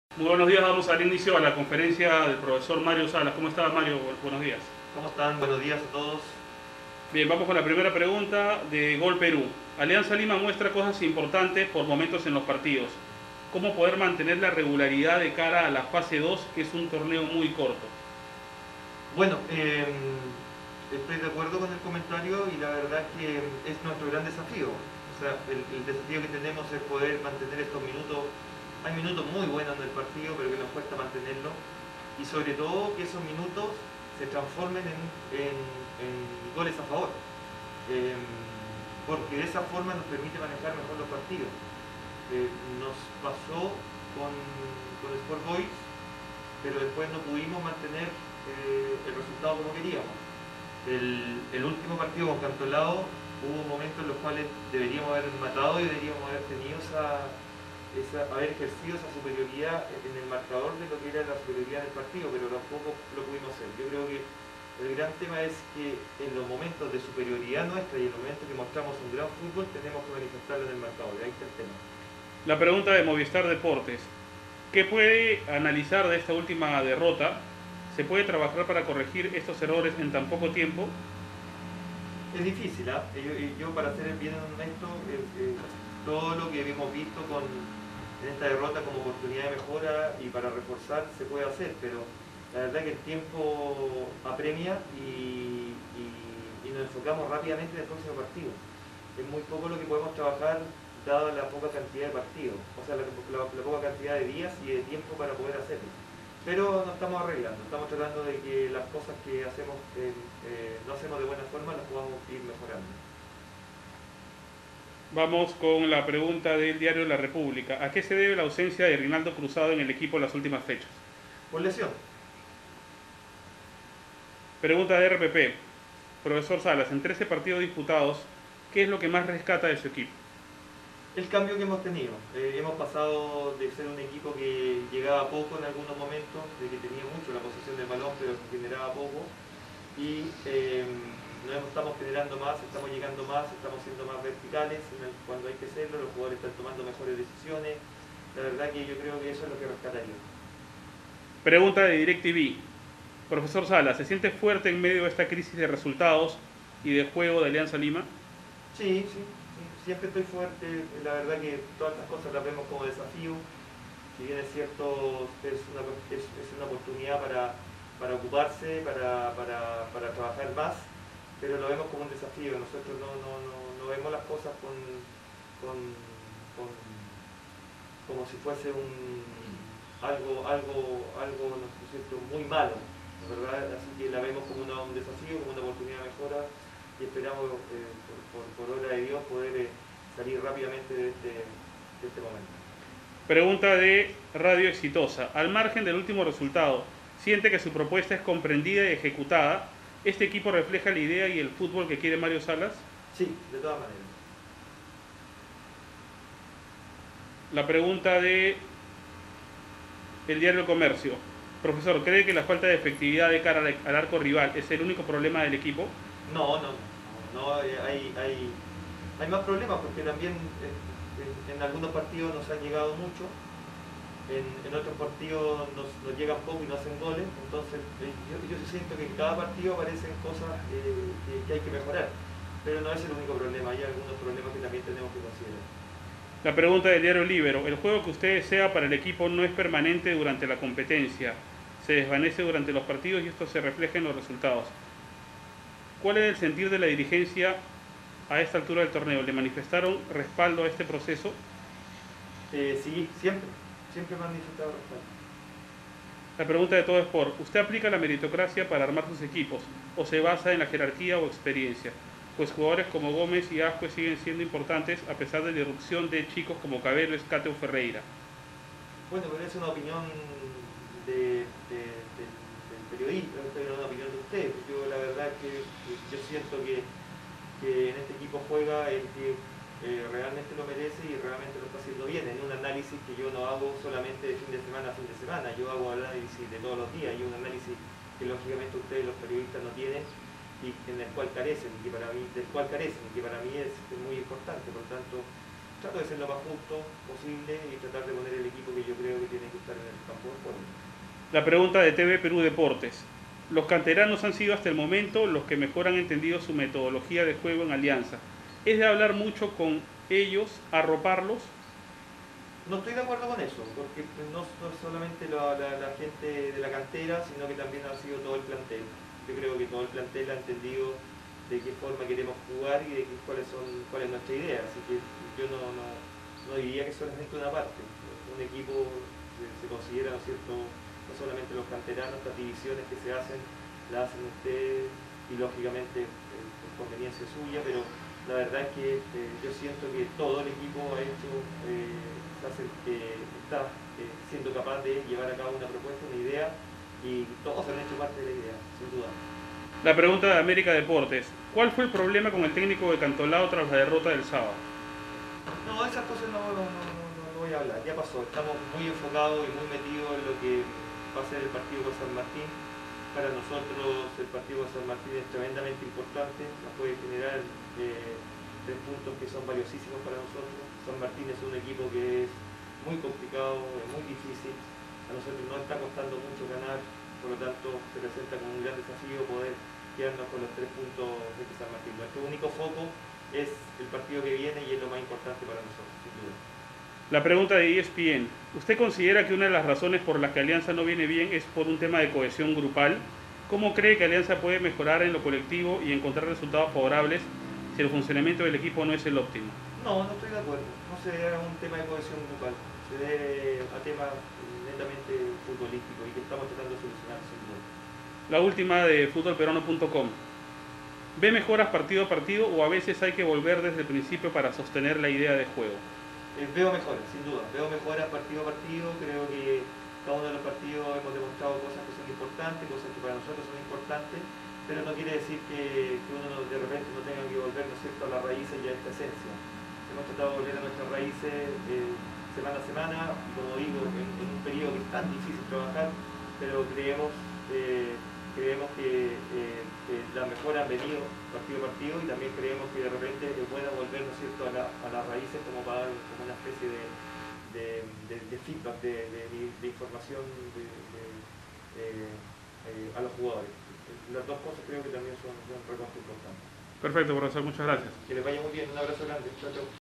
Mario Salas: "Nos esforzamos cada día para conseguir los resultados que queremos"
DT Alianza Lima